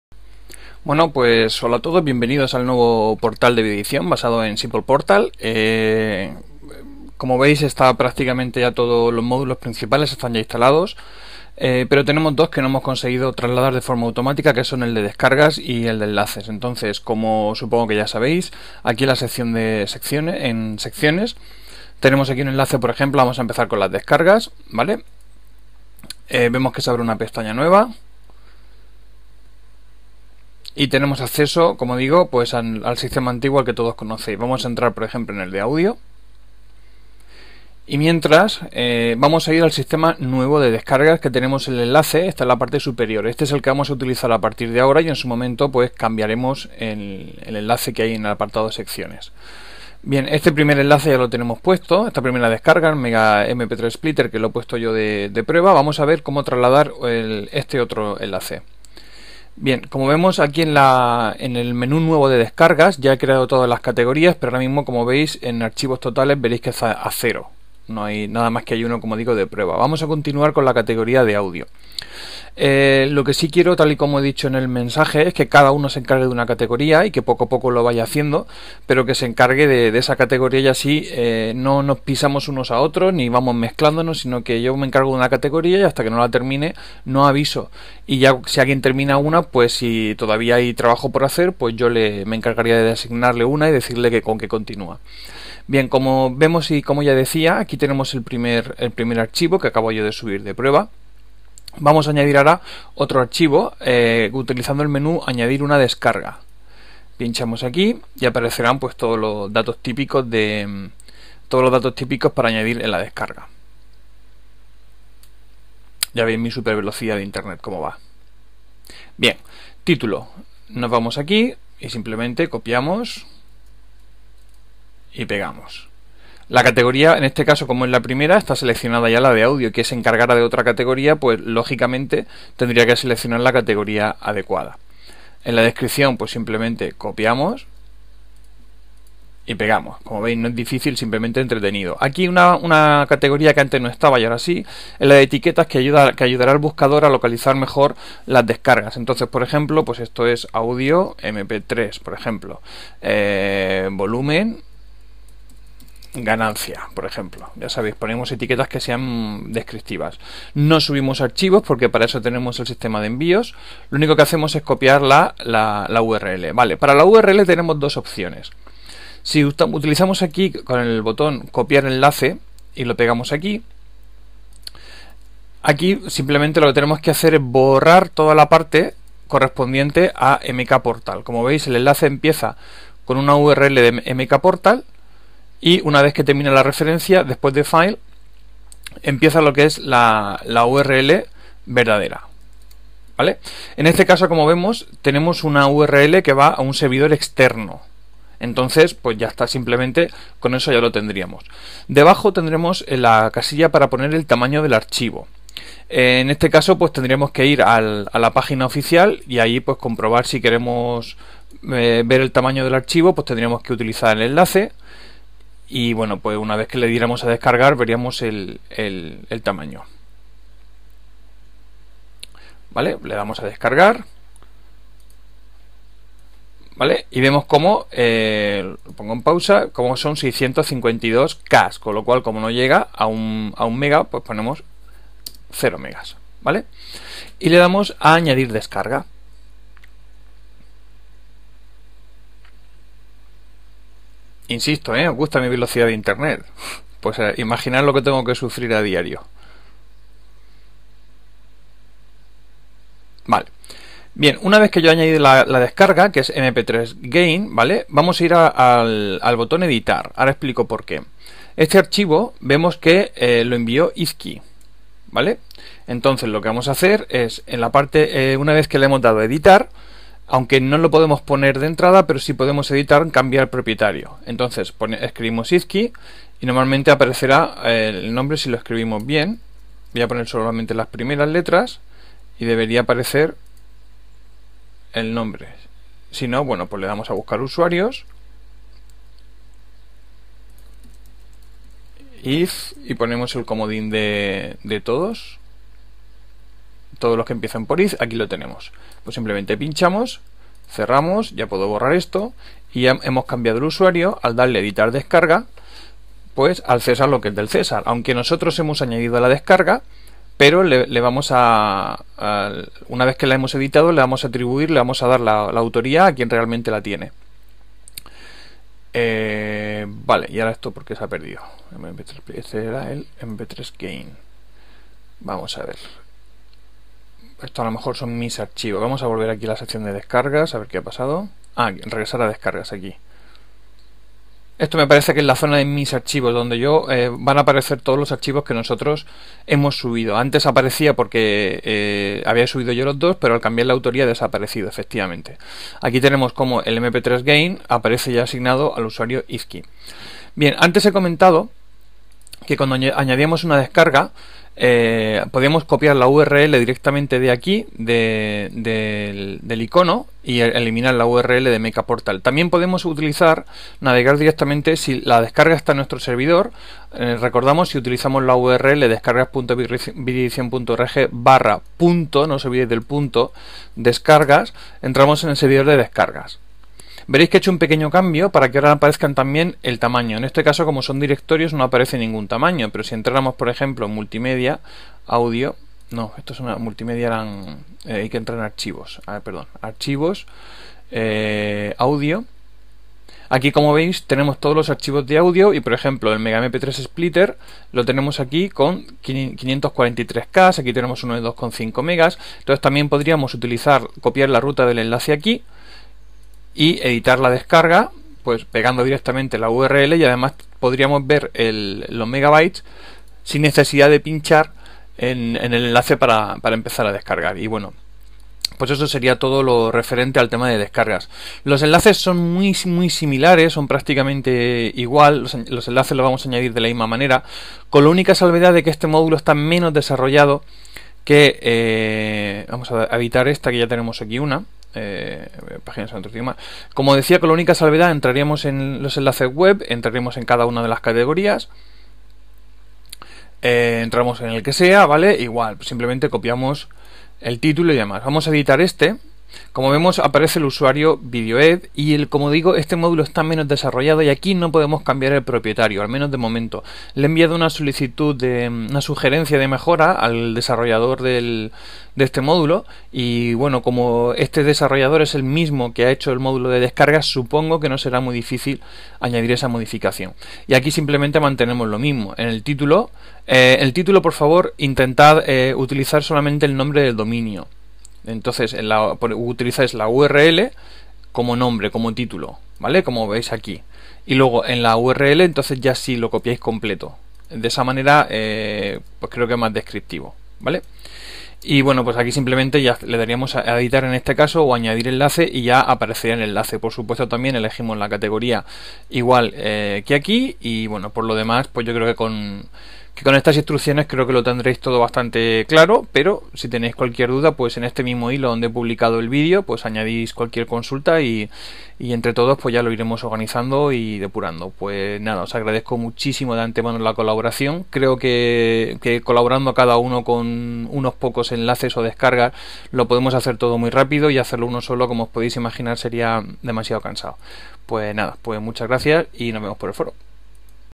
Traspaso de enlaces y descargas a videoedicion.org 6.x
Este vídeo, destinado a colaboradores de videoedicion.org, explica cómo hacer el traspaso de los enlaces y descargas del portal anterior (MKPortal) al actual (SimplePortal)